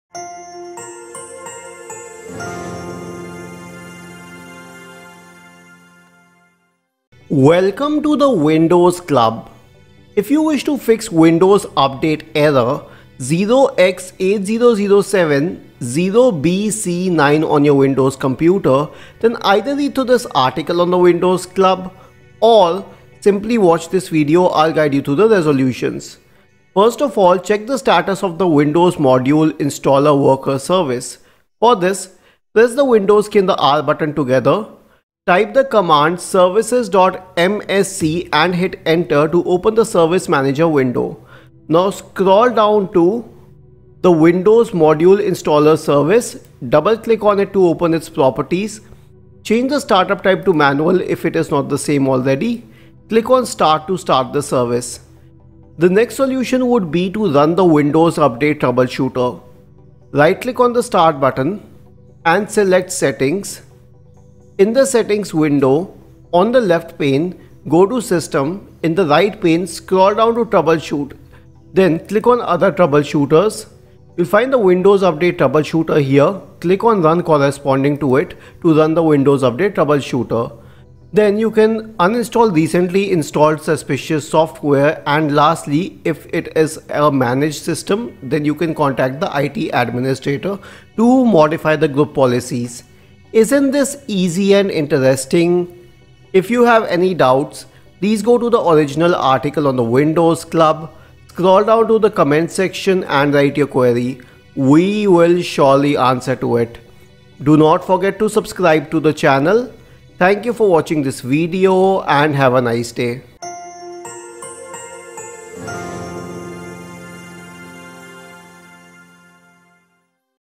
Fix Windows Update error 0x80070BC9 on Windows
Windows Updates are necessary for every Windows system. They need to be installed regularly, even more so because they include security updates crucial to the system. But at times, Windows Update fails and gives out error code 0x80070BC9.

0x80070BC9 – ERROR_FAIL_REBOOT_REQUIRED. The requested operation failed. A system reboot is required to roll back changes made.

Here are a few fixes and suggestions:

1] Check the status of Windows Modules Installer Worker Service

2] Run the Windows Update Troubleshooter

3] Uninstall recently installed software

4] Contact the IT administrator to modify the group policies

Timecodes:
0:00 Intro
0:26 Check status of Windows module Installer service
0:56 Run Windows Updates troubleshooter
1:20 Uninstall recently installed software